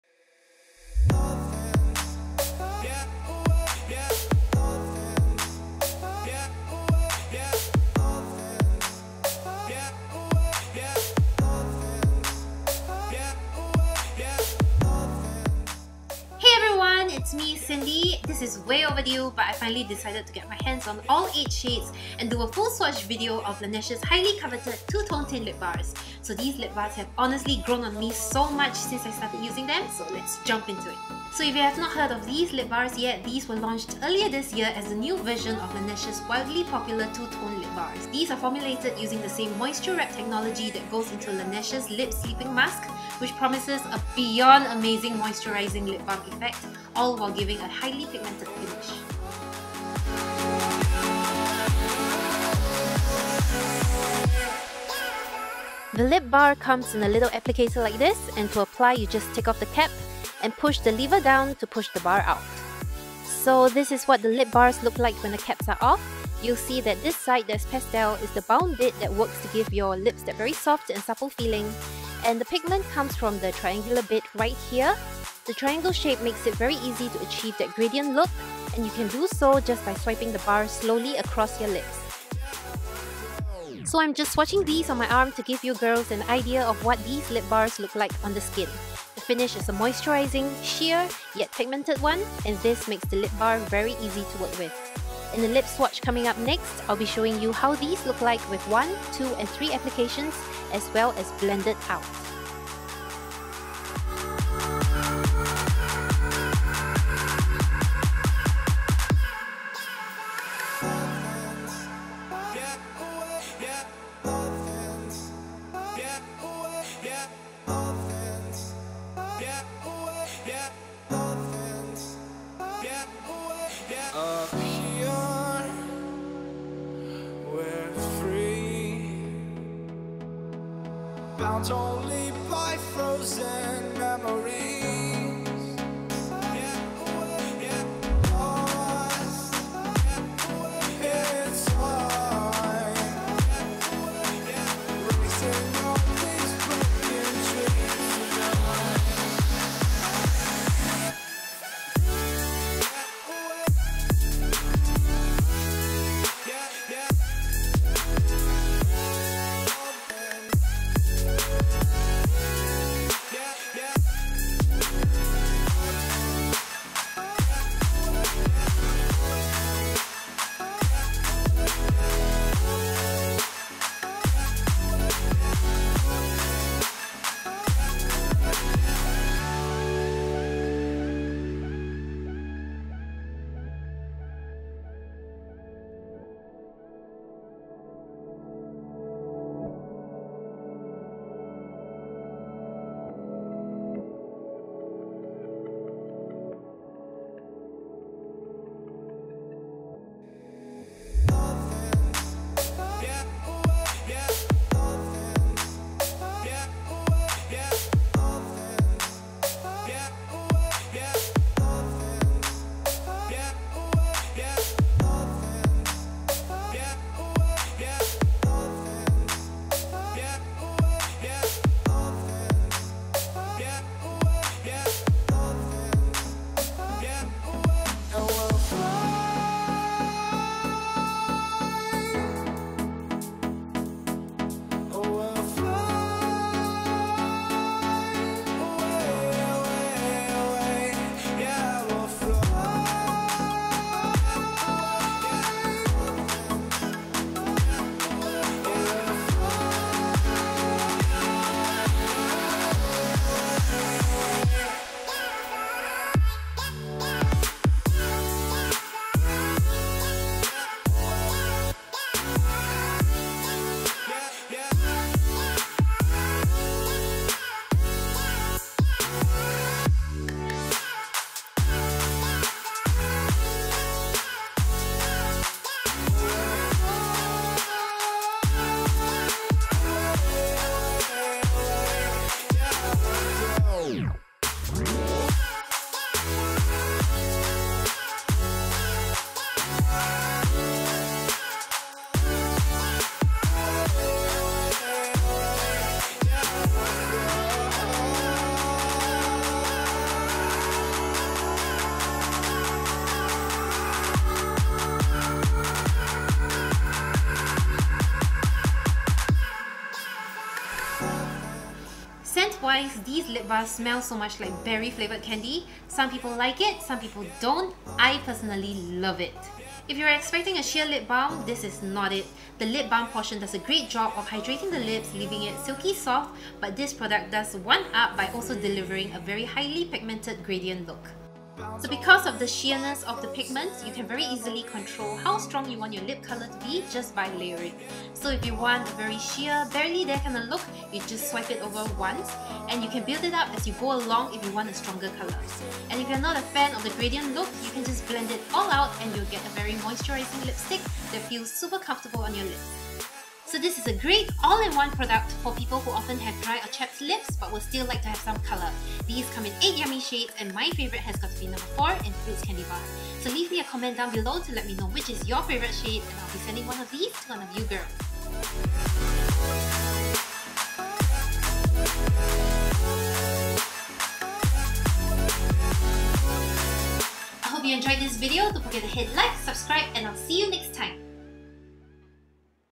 LANEIGE Two Tone Tint Lip Bars - ALL 8 Shades! Lip Bar Review & Lip Swatches
Hey girlies! Back with a new Laneige Two Tone Tint Lip bar video because I felt like my first video was not doing these awesome Laneige Two Tone Tint Lip Bars any justice! In this video, I will be arm and lip swatches for all 8 shades and giving my honest thoughts on this product!

I'll be giving away one of these lip bars (well, I'll purchase a new one of course!) to one subscriber!

*Contest is open for SEA countries! Namely Singapore, Malaysia, Vietnam, Thailand, Indonesia, Philippines, Brunei, Cambodia.

R E L A T E D V I D E O

PRODUCTS

1. Laneige Two Tone Tint Lip Bars in 01 Cotton Candy
2. Laneige Two Tone Tint Lip Bars in 02 Tangerine Slice
3. Laneige Two Tone Tint Lip Bars in 03 Tint Mint
4. Laneige Two Tone Tint Lip Bars in 04 Fruits Candy Bar
5. Laneige Two Tone Tint Lip Bars in 05 Apple Jell-o
6. Laneige Two Tone Tint Lip Bars in 06 Tomato Sherbet
7. Laneige Two Tone Tint Lip Bars in 07 Lollipop Red
8. Laneige Two Tone Tint Lip Bars in 08 Cherry Milk